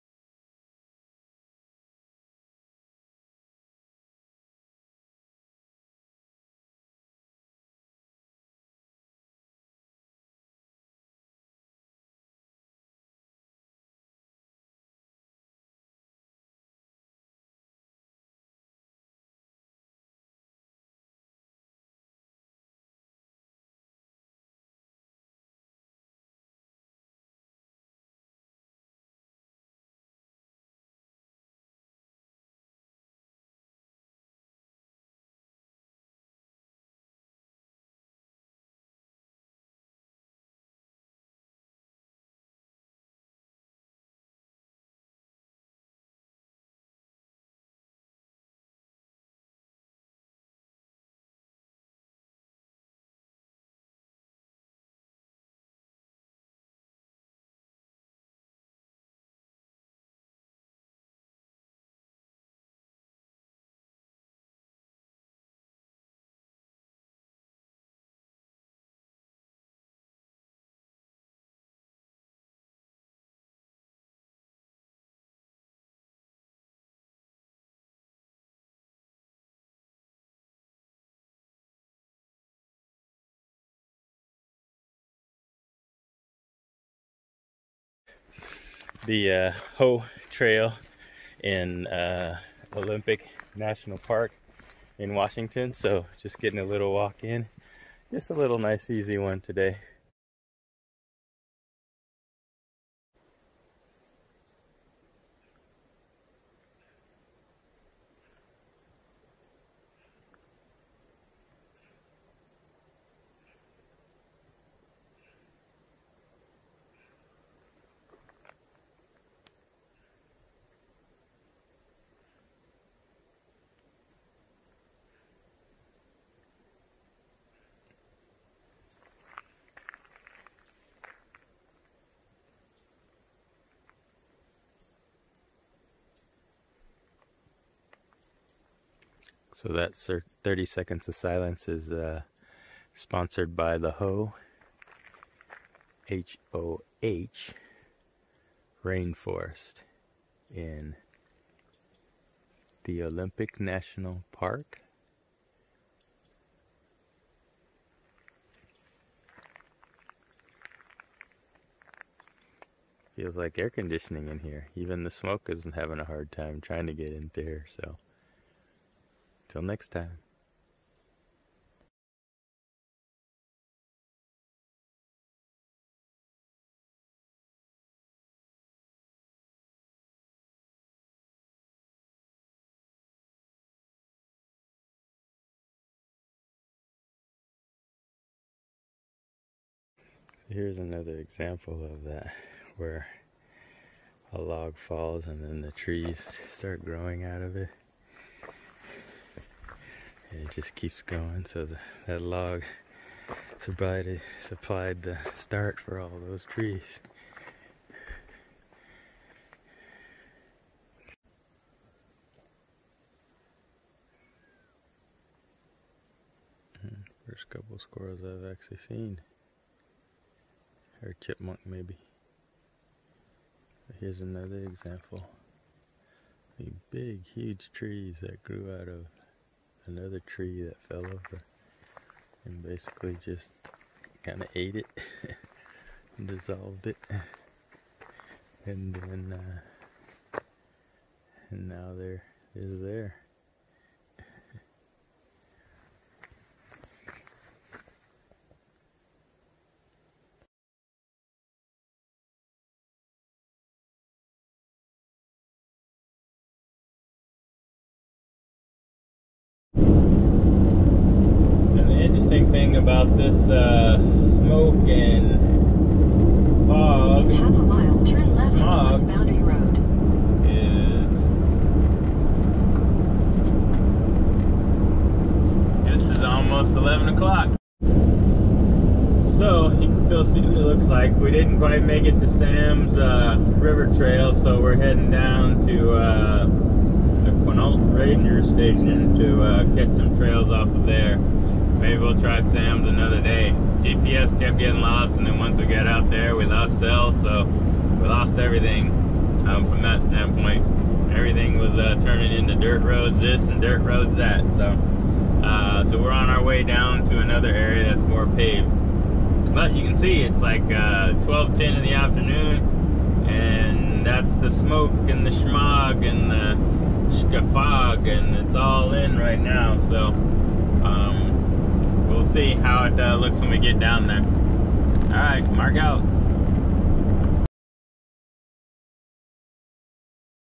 Olympic Pt1
Road views of Traveltown Mark road trip national park in camper van tent Olympic National Park Washington